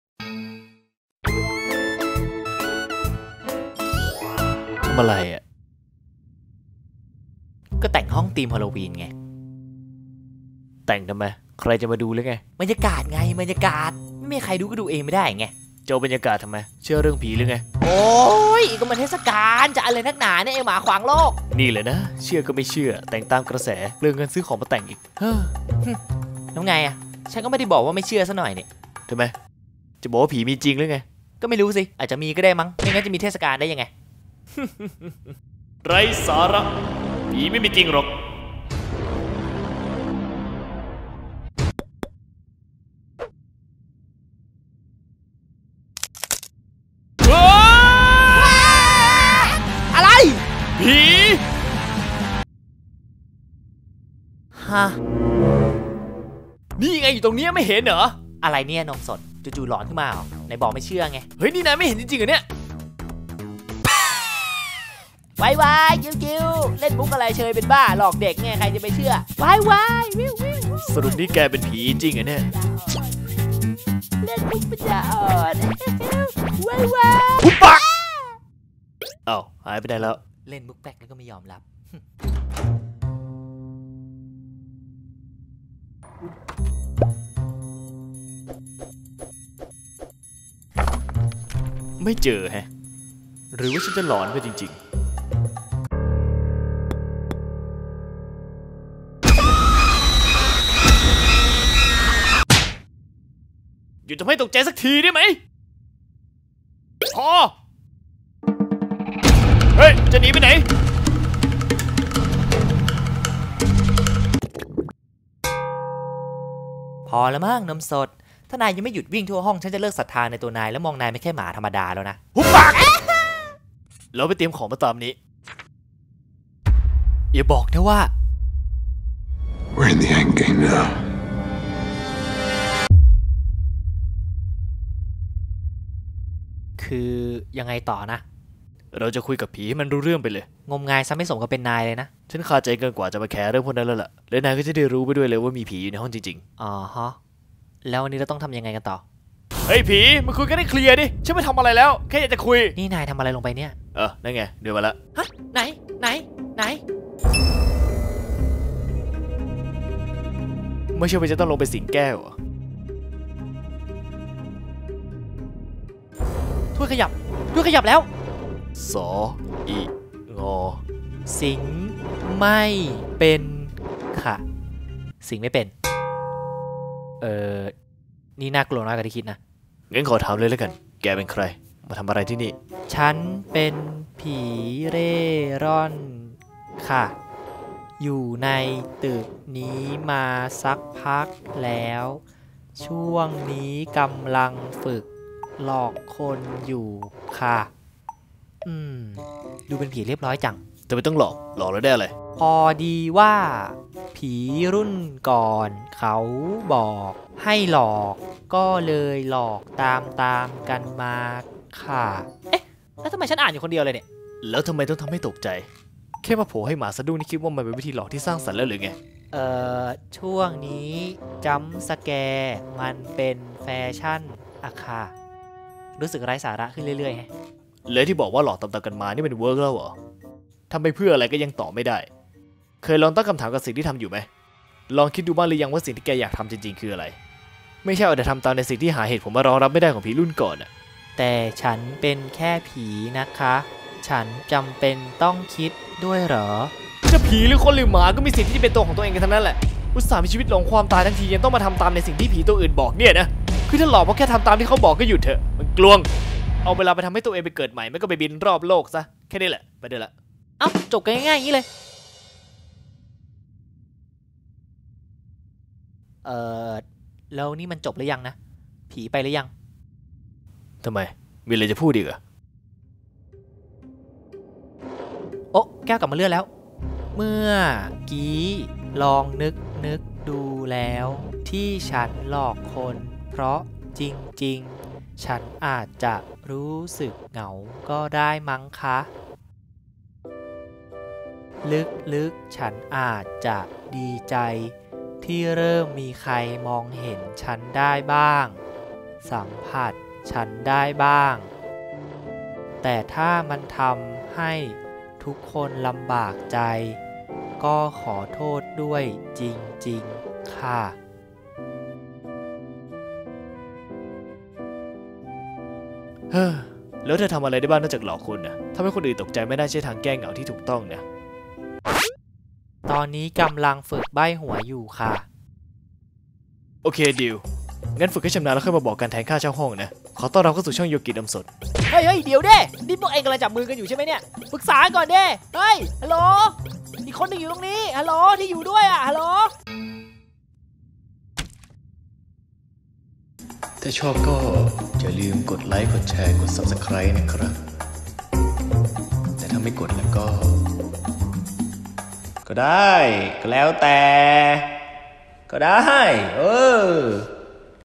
YOGI & NOMSOD's STORY | EP.4 - วันผีสะกิดหัวใจ
Happy Halloween!! 💀 🎃 👻 (แบบเลทๆ)
เมื่อโยกิ และนมสดพบว่าดันมีผีอยู่ในห้องซะงั้น?! และคลิปนี้จะสะกิดหัวกันขนาดไหนไปรับชมกันได้เล้ย~

ติดตามช่องทางอื่นของพวกเราได้นะ